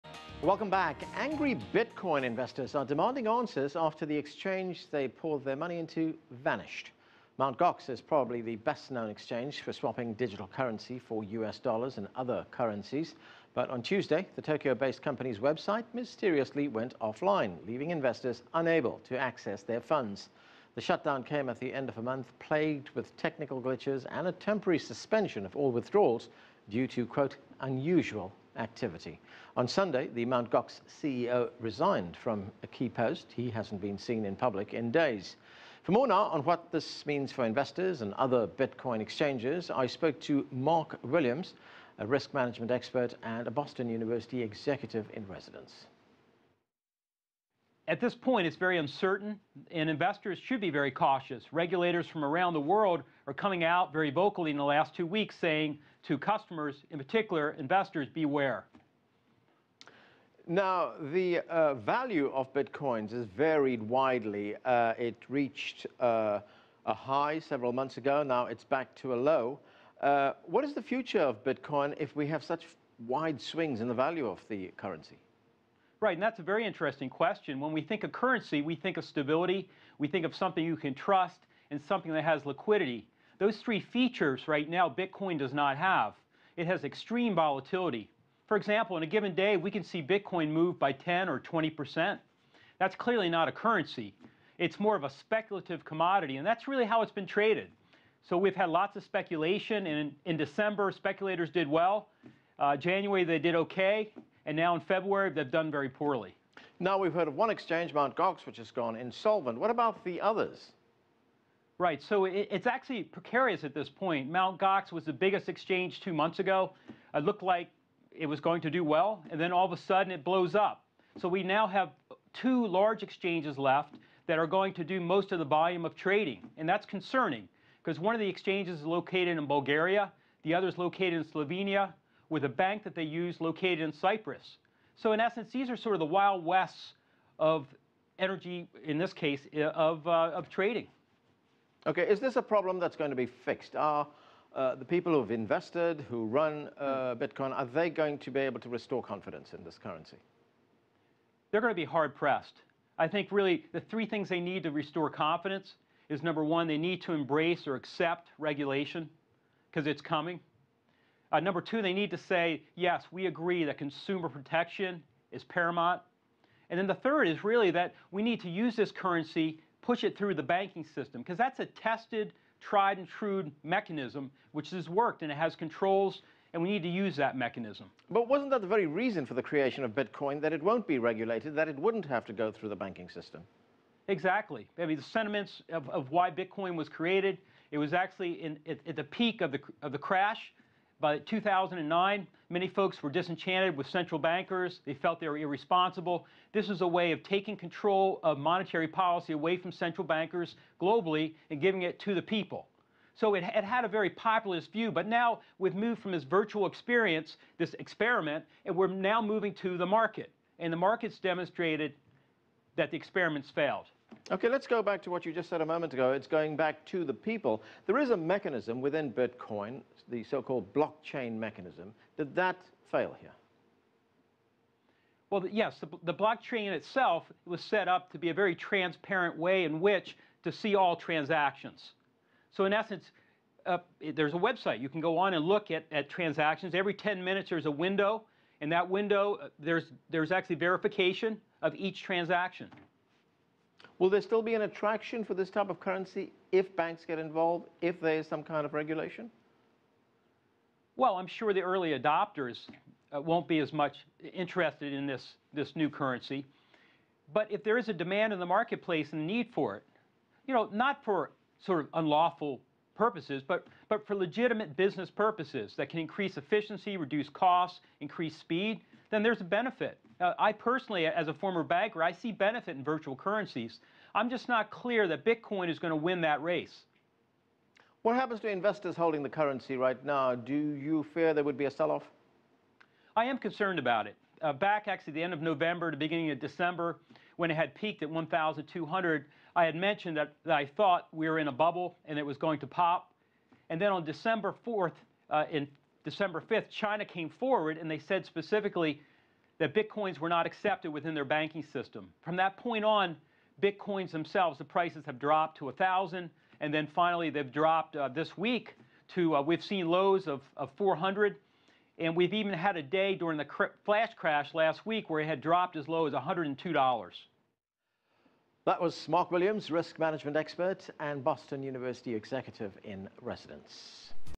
Major Bitcoin Exchange Goes Offline
Mark Williams, risk management expert and a Boston University executive-in-residence discusses the consequences on the lack of Bitcoin regulations.